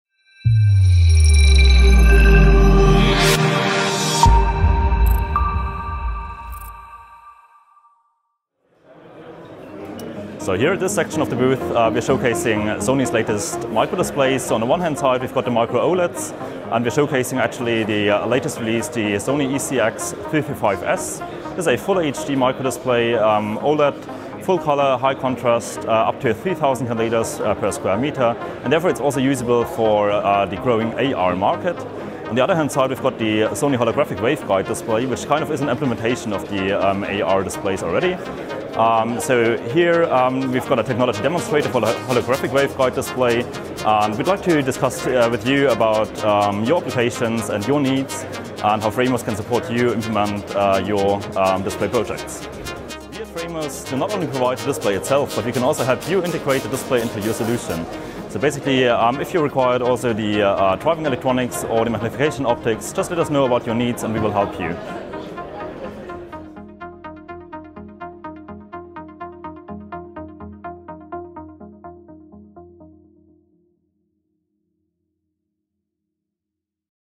OLED Microdisplays | FRAMOS Virtual Exhibition
Live demo showing a video loop on the tiny display surface.

The video is streamed from a standard laptop, however this could be any kind of HDMI device. The system highlights the ease of evaluating and integrating the latest microdisplay technology.